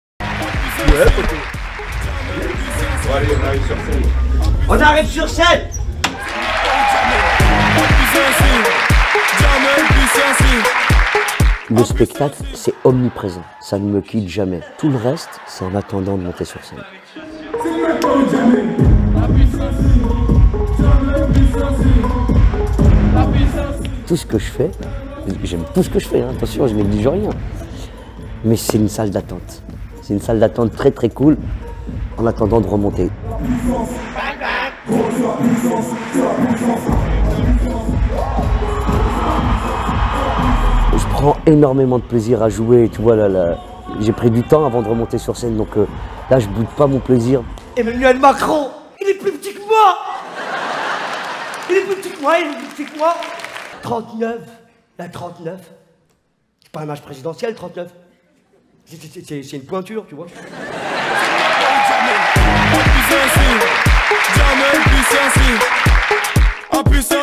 Jamel Debbouze vous donne rendez-vous le dimanche 11 mars
Jamel Debbouze, qui remonte sur scène après six ans d'absence, s'est longuement confié devant les caméras des journaux du groupe EBRA. Rendez-vous dimanche 11 mars sur nos écrans et dans nos colonnes.

Entretien : Paul-Marie PERNET
Images et Réalisation : Jean-Emmanuel LAGES
Mars 2018 - EBRA